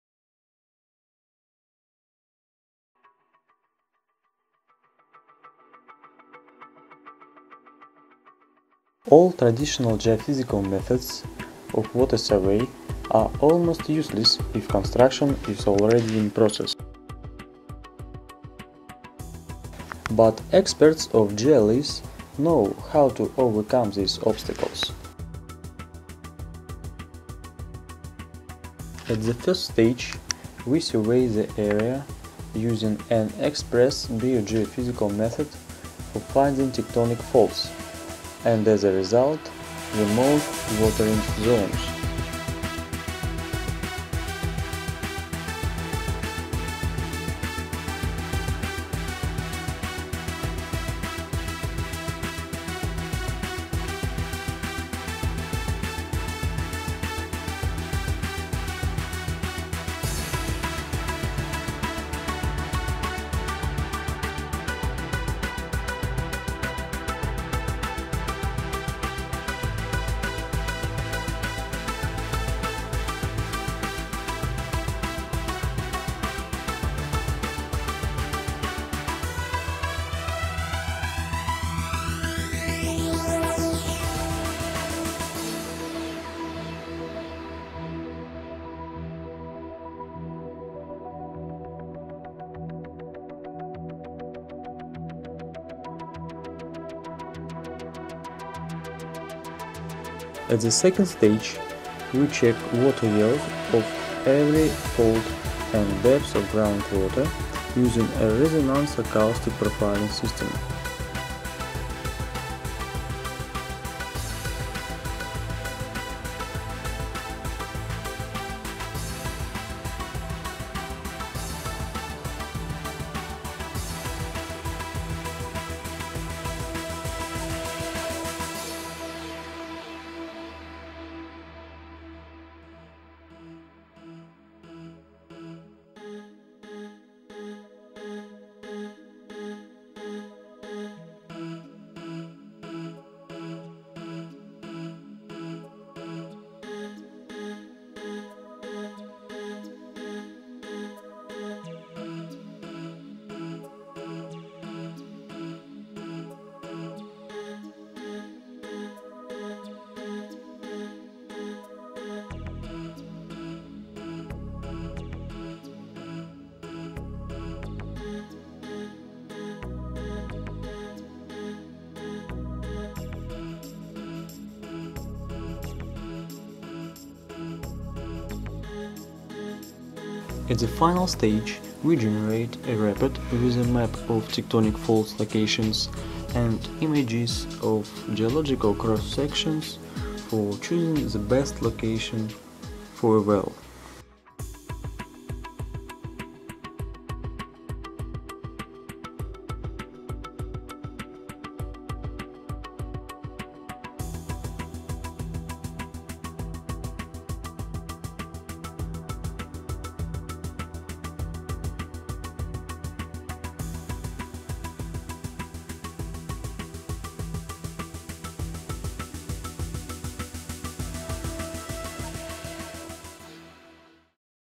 water survey
All traditional geophysical methods of water survey are almost useless if construction is already in process. But specialists of GEOLISS know how to overcome these obstacles.
At the first stage we survey the area using an express bio-geophysical method for finding tectonic faults and, as a result, the most watering zones.
At the second stage we check water abundance of every fault and depth of groundwater using a resonance acoustical profiling system (RAP).
At the final stage we generate a report with a map of tectonic faults locations and images of geological cross-sections for choosing the best location for a well.